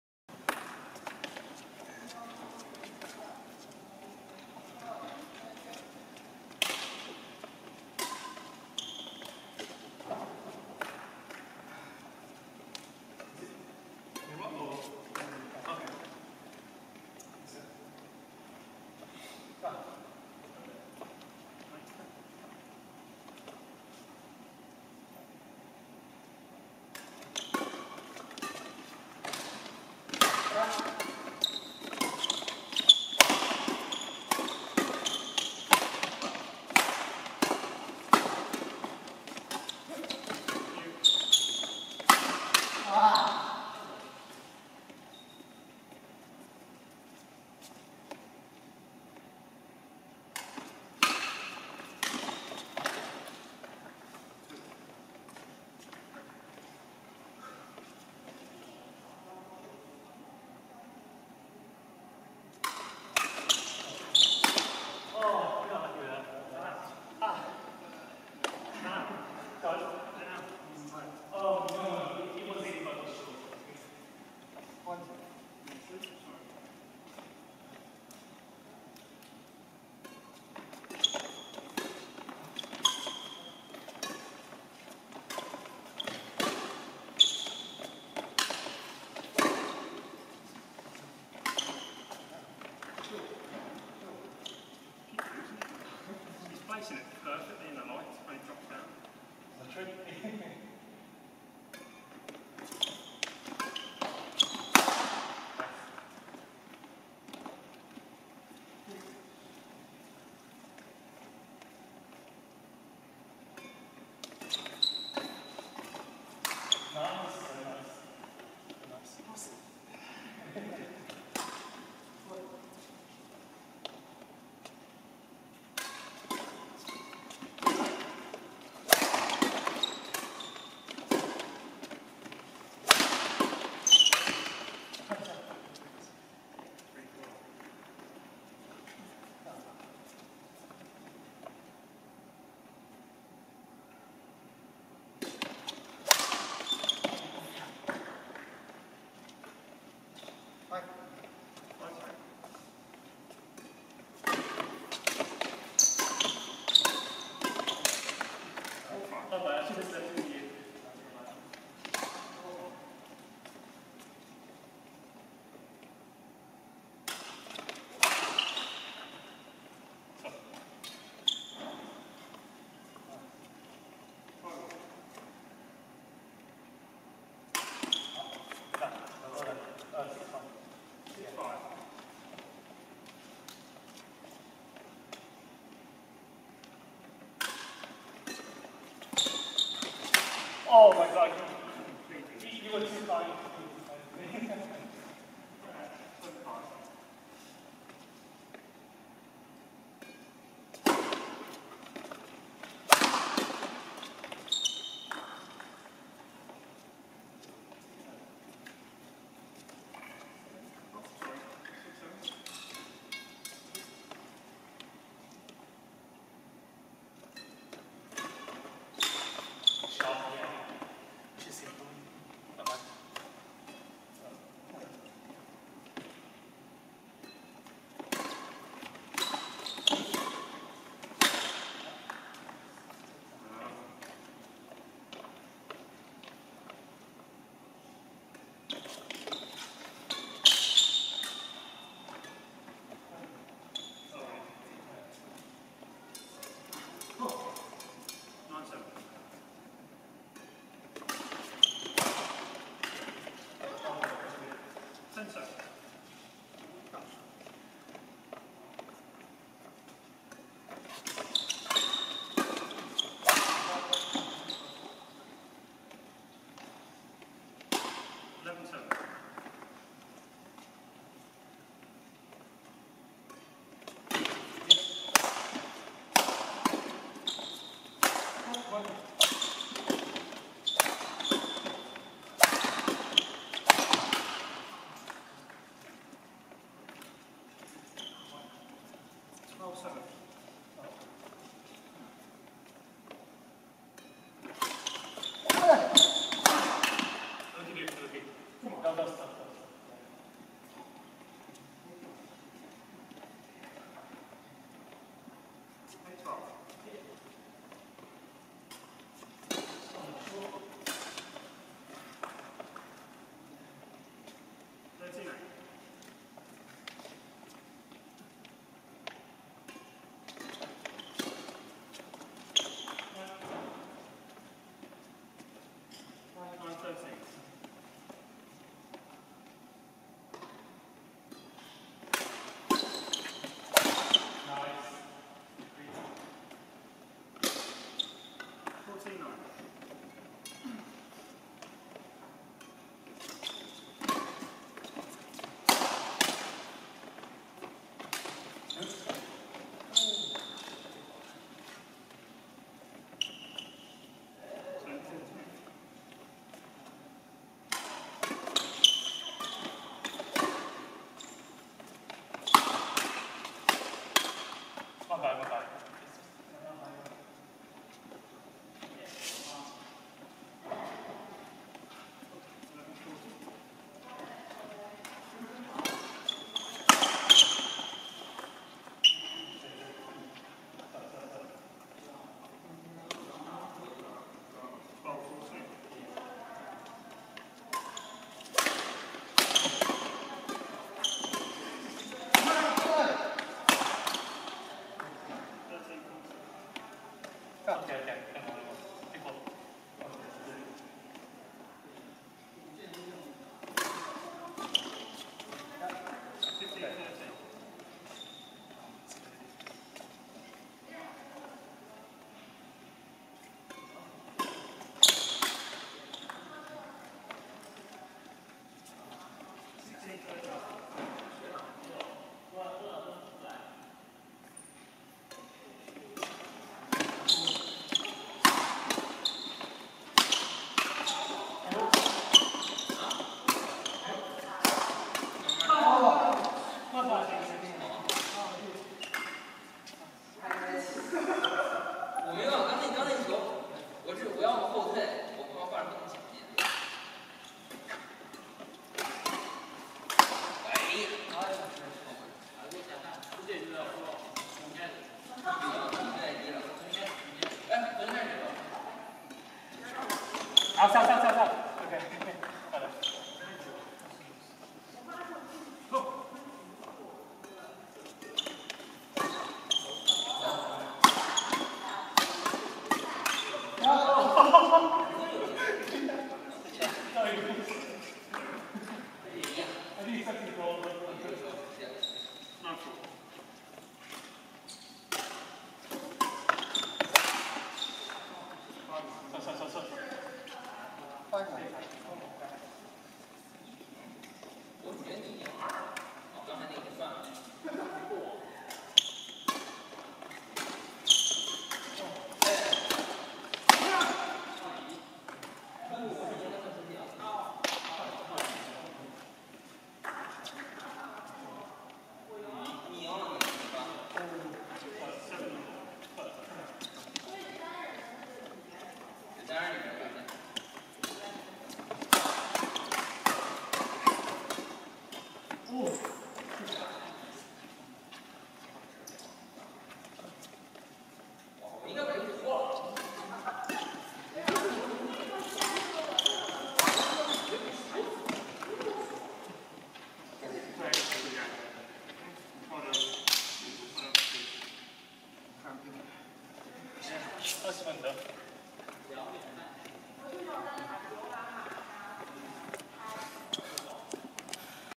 Dan/Jason vs Keat/Rob Final Game
Time Stamp: 16/08/2019 - 3pm-5pm
Location: The Sir Doug Ellis Woodcock Sports Centre
Winners: Dan/Jason